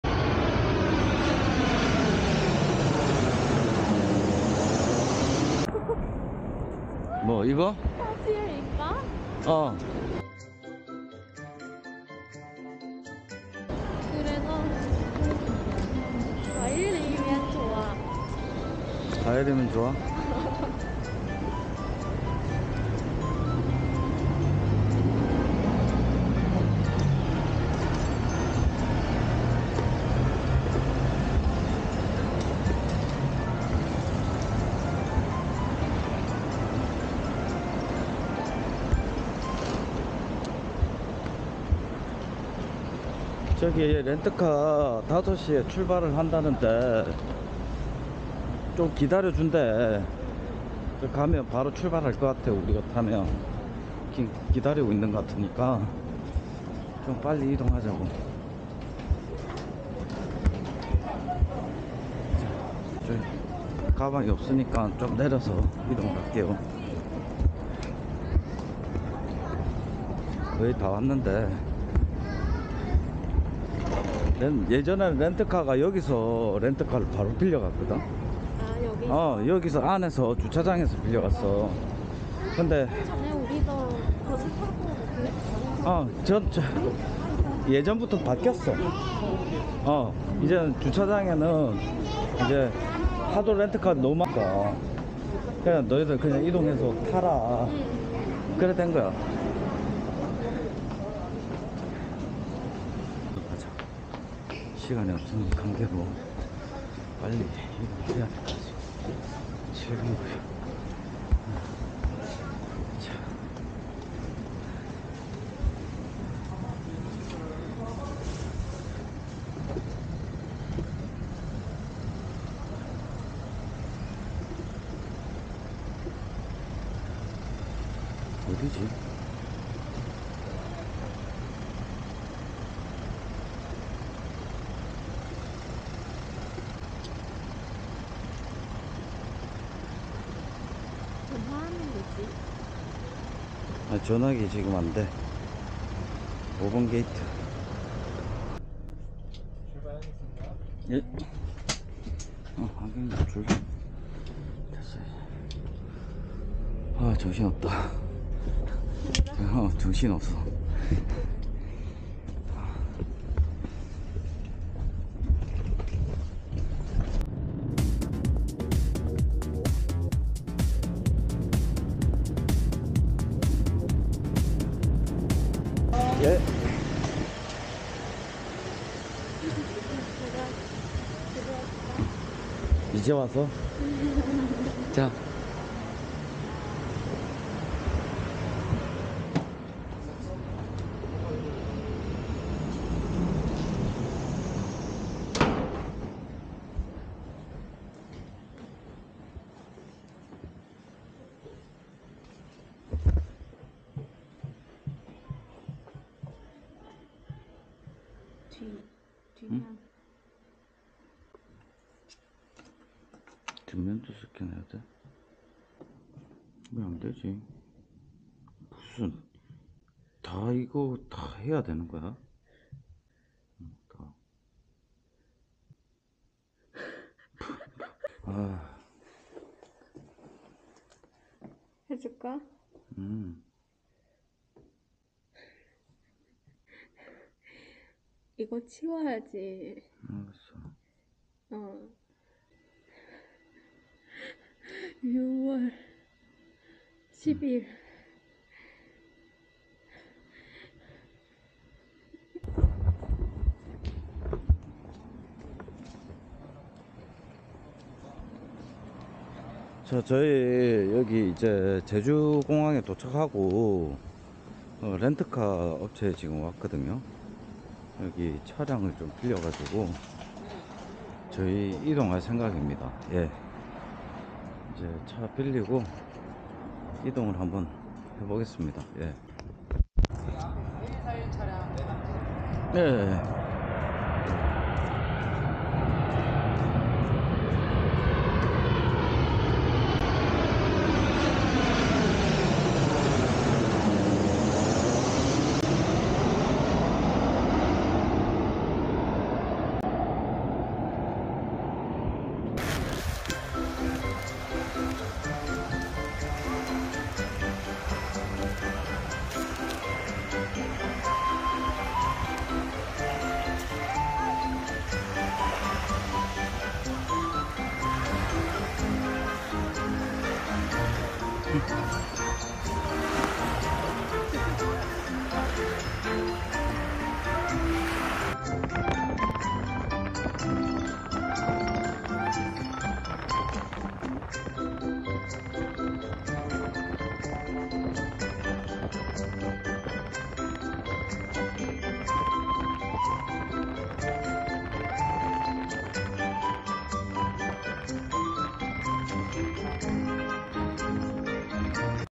[국제커플/한베커플] "베트남 여자친구 zelle랑" 🇰🇷 🇻🇳 렌트카 타고 제주도여행 시작!! (2편) Cặp đôi Việt Nam Hàn Quốc
[국제커플/한베커플] 베트남 여자친구 zelle랑 렌터카 타고 제주도여행 출발 Cặp đôi Việt Nam Hàn Quốc

여자친구와 행복한 시간 보냈네요.

항상 즐겁고 행복한 날들만 가득하길 .

Cặp đôi Việt Nam Hàn Quốc (한베커플/국제커플)

* 이 영상은 어떠한 경우에도 공식적인 허가 없이 영상의 일부 또는 전체를 복제, 전송, 배포하거나 변경하여 사용할 수 없습니다.